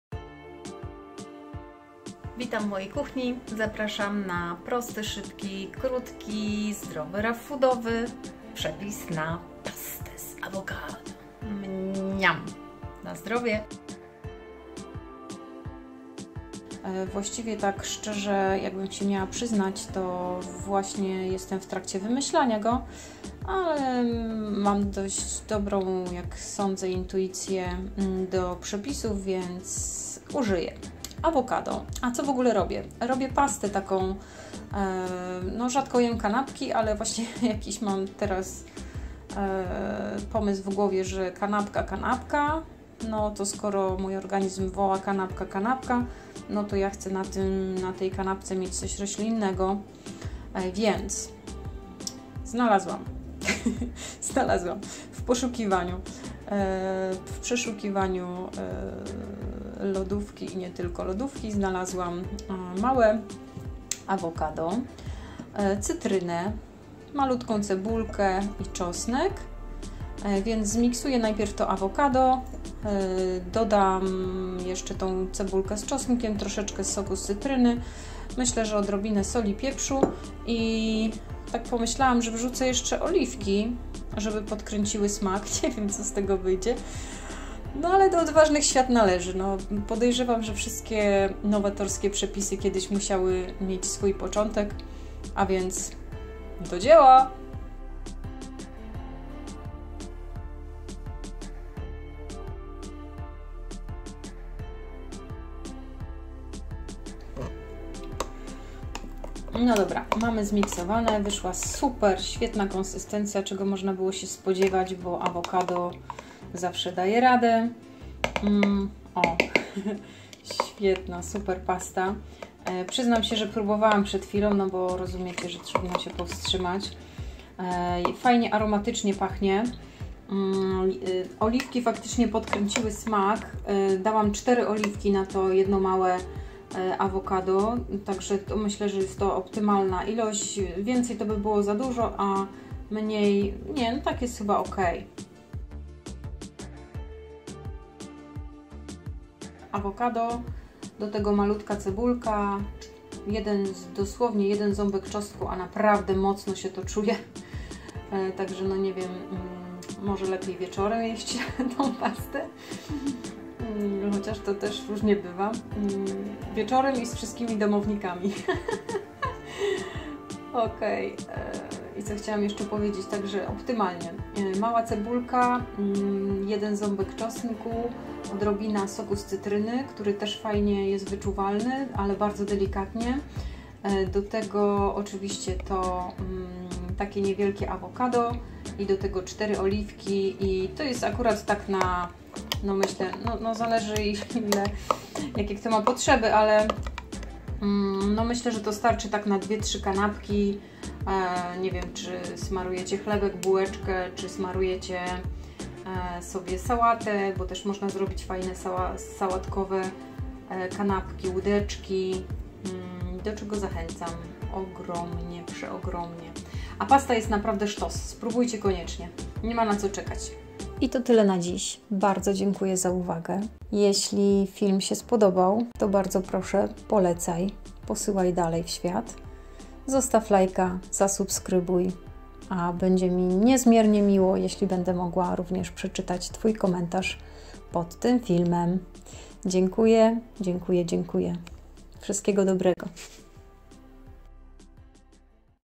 PASTA z AWOKADO * RAW food * VEGAN
Krótko o nowym przepisie na fajną pastę. Szybko, zdrowo i smacznie!